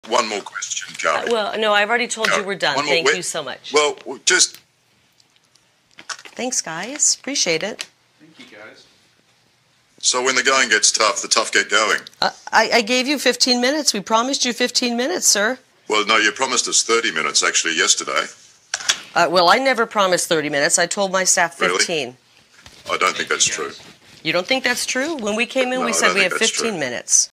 The Fake News Media Does Not Know How To Handle Kari Lake
Kari Lake takes it to the fake news media and they do not know how to handle her 😂😂 @LawAndCrimeNews